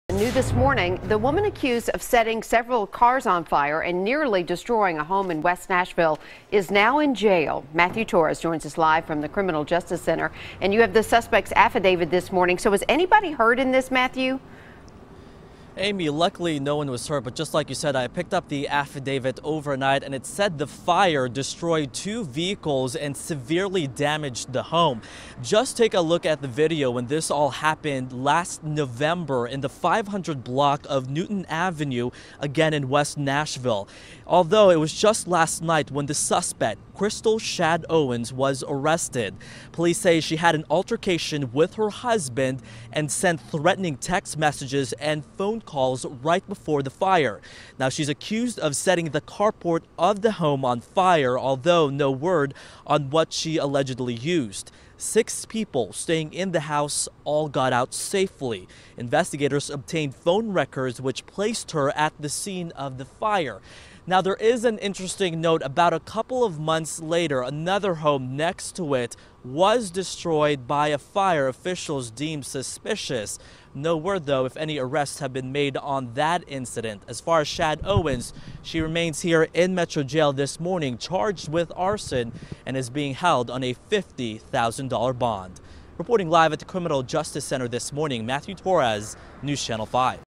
Woman Arrested In November Arson
A woman has been taken into custody in connection with an arson from November.